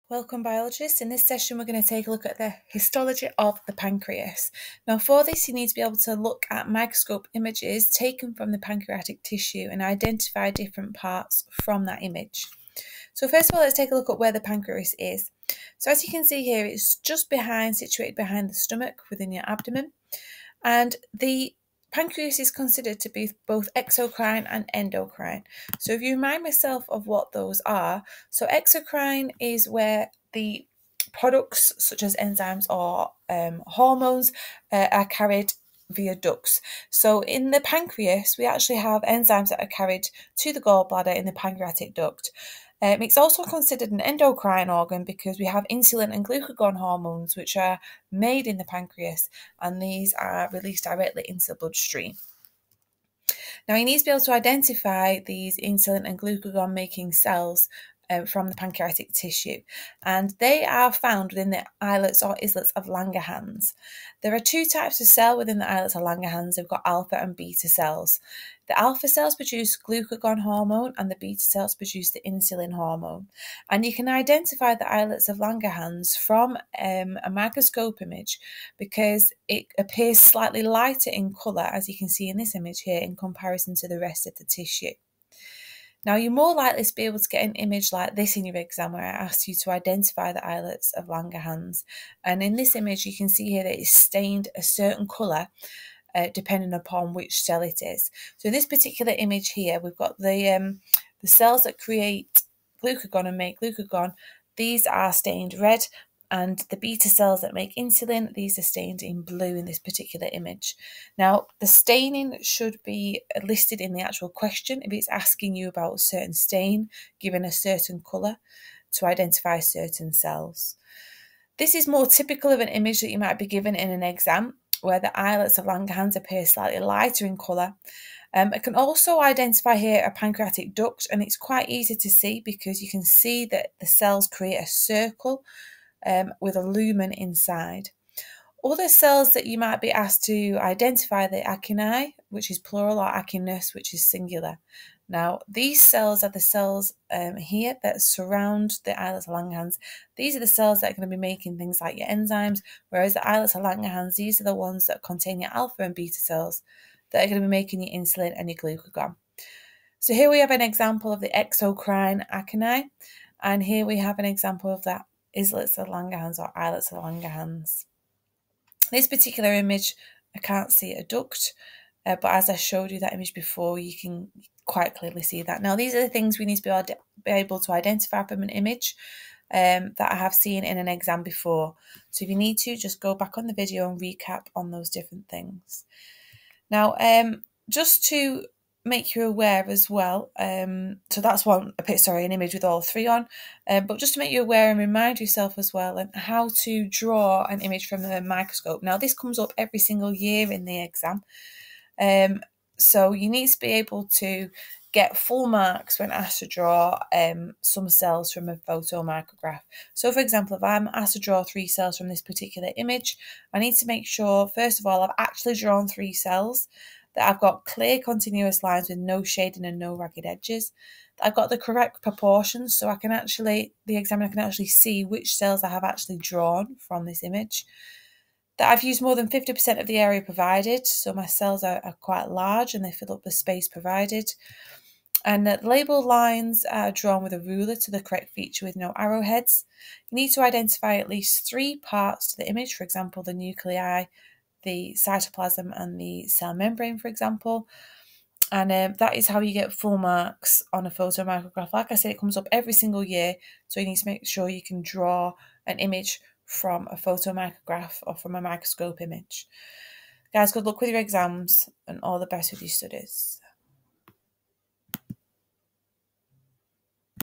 5.1.4 Hormonal Communications c) i)The histology of the pancreas ii) Examination of stained tissue
OCR A level Biology
5.1.4 Hormonal Communications
c) i)The histology of the pancreas
ii) The examination and drawing of stained sections of the pancreas to show the histology of the endocrine tissues.